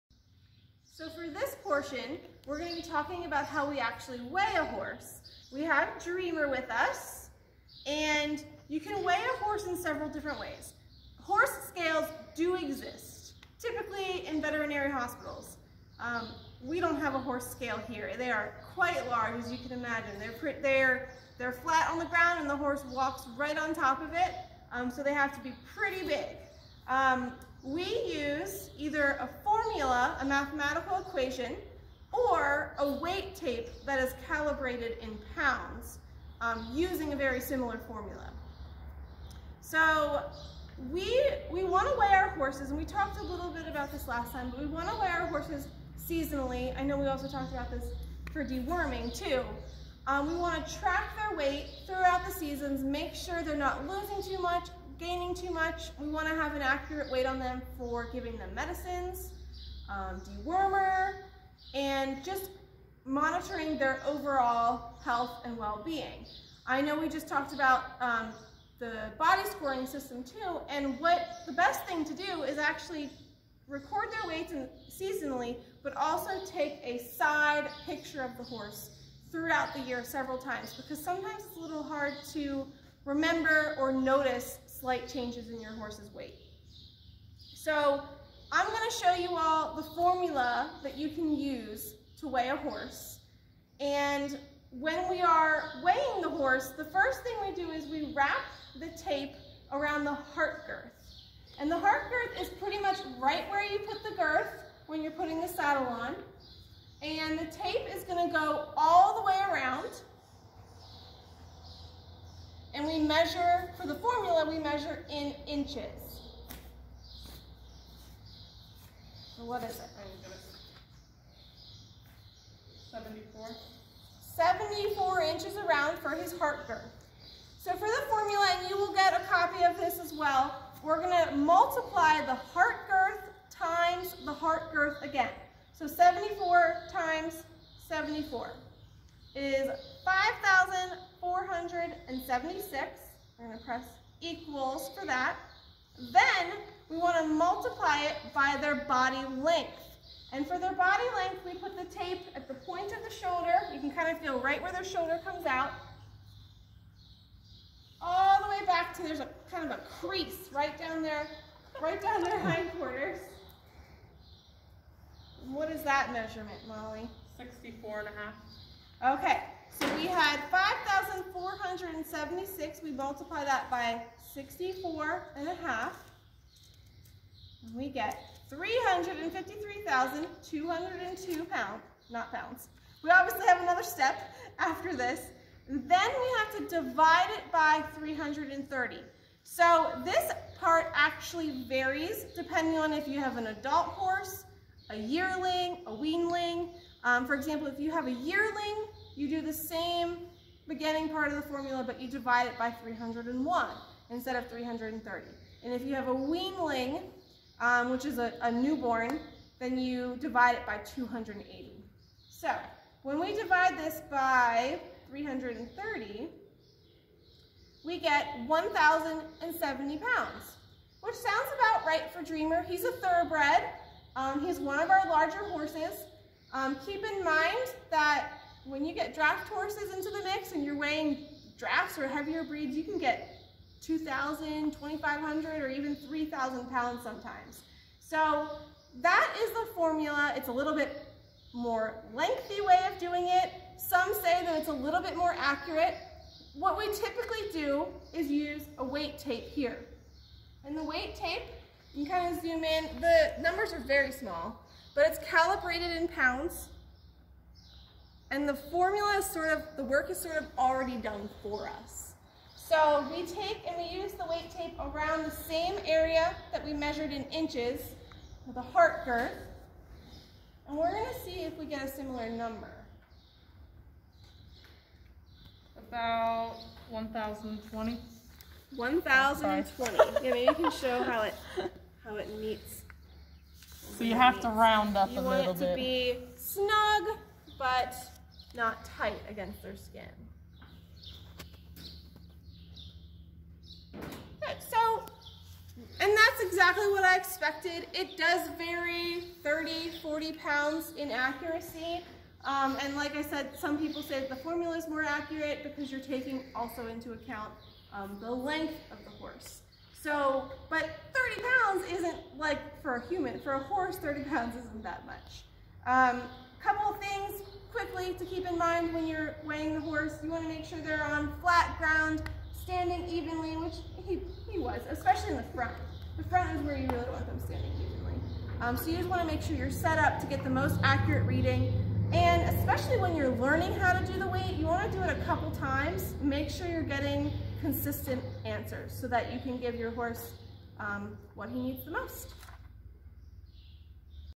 Week #9/Video #5 - Equine Assisted Learning - Weighing a Horse
Megan demonstrates how a horse's weight is estimated.